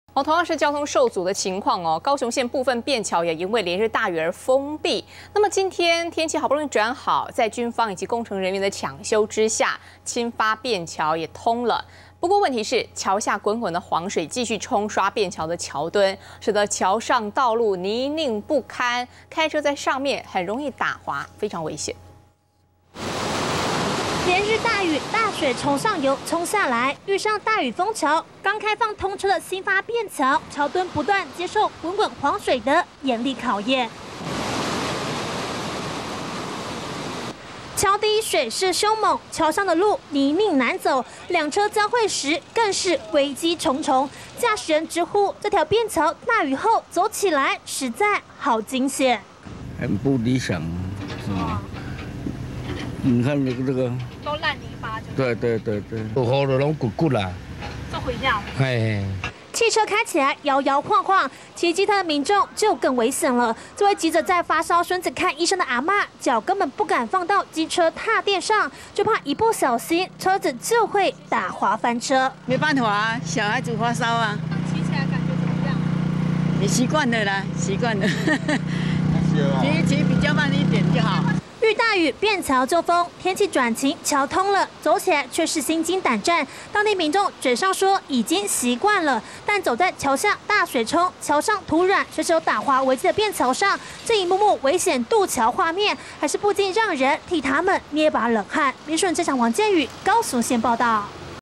新發便橋搶通 民眾冒險通行－民視新聞
【民視即時新聞】連日大雨，造成高雄縣部分便橋封橋，今天的天氣轉好，在軍方以及工程人員搶修下，新發便橋是通了，但橋下滾滾黃水沖刷便橋橋墩，橋上道路是泥濘難走，駕駛人直呼，走起來是膽顫心驚，實在好危險。